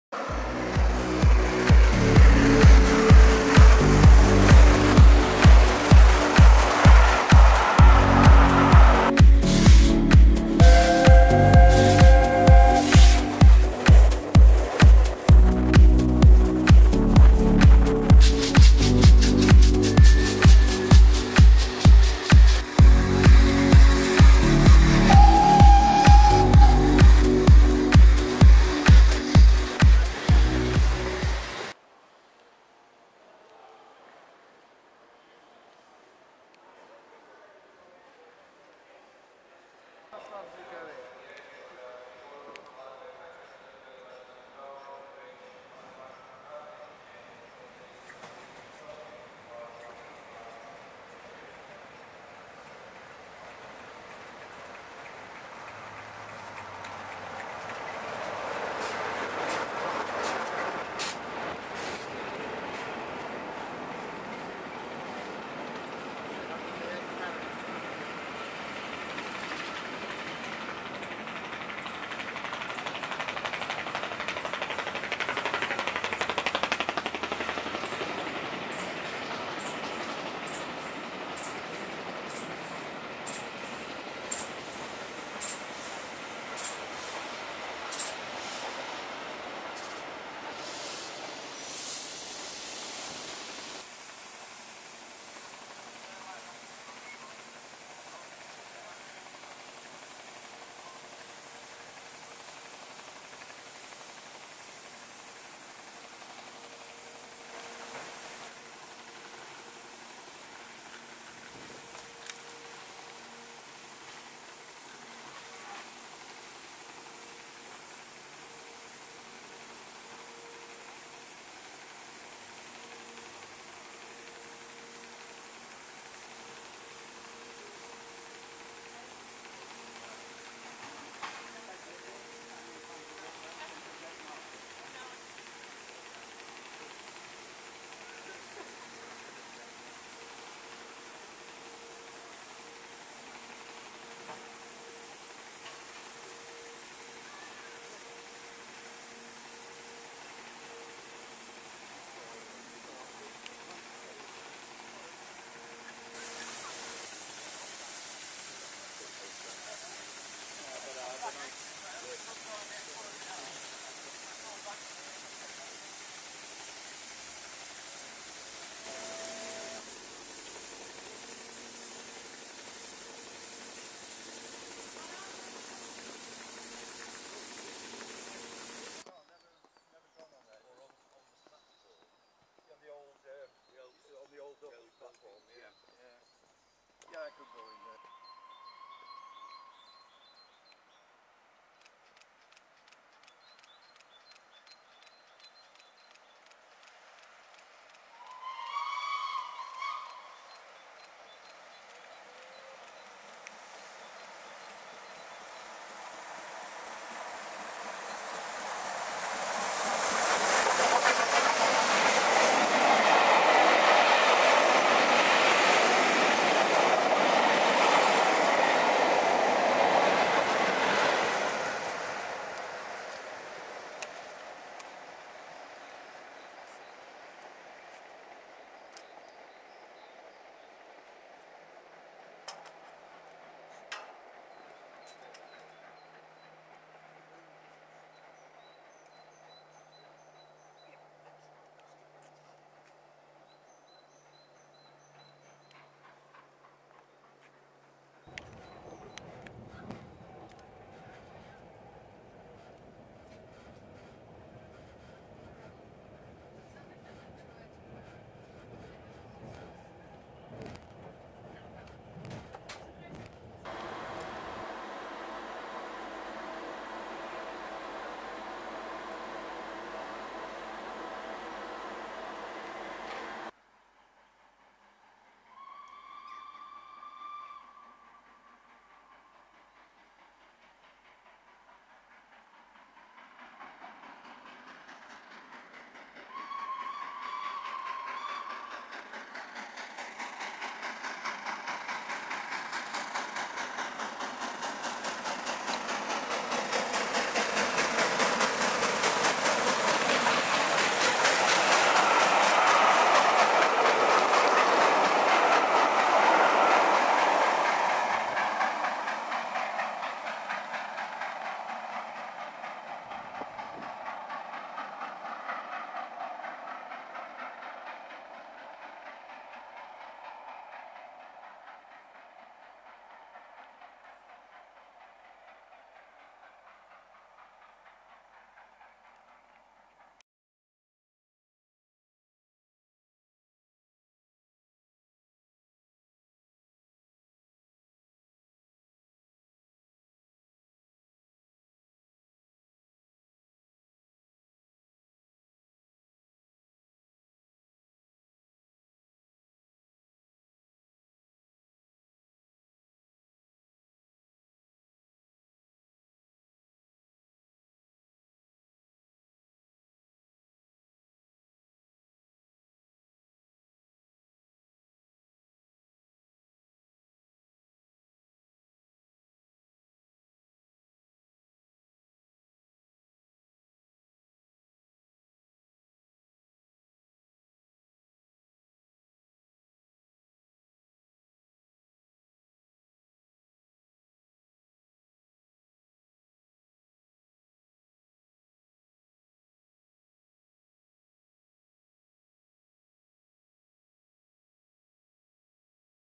61994 The Great Marquess 1Z56 The West Highlander D1 Friday 19th September 2014
On a nice warm Friday morning I was racing steam again.

On today's program we race John Cameron's ex LNER K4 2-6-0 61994 The Great Marquess as she heads day 1 of Railway Touring Company's West Highlander from Preston to Glasgow Central via Shap and Beatock.

We start at Preston were we see 61994 arriving with the coaching stock behind newly repainted west coast 47 spoon 47245 after bringing the stock and the K4 in from Carnforth steamtown that morning.

We have a look round the K4 as she waits for her passengers to board the train. Surprisingly there was no standard class coaches on the 7 coach train mostly first class and premier class.

We leave before the K4 in order to get our 1st of 2 speeding shots. Our first speed shot is at Lancaster just 10 miles away from Carnforth were we see her whistling past (apologies for the shakiness of the camera for that bit). We past the trip at Carnforth as we went past 47245 was getting uncoupled from the train.

Our final location of filming The West Highlander tour was at Oxenholme Lake District were we see her whistling past the station and went through like a riot as she climbed the hill through he station on her way up to Shap.

To sum up it was an awesome morning getting 61994 on the mainline after not seeing her on the national network for 2 months and this was her first mainline trip since she failed at Llandudno Junction while working the return leg of the Welsh Mountaineer trip back in July earlier this year (2014). Anyway it was great to see her back on the mainline.

Thanks for reading enjoy the film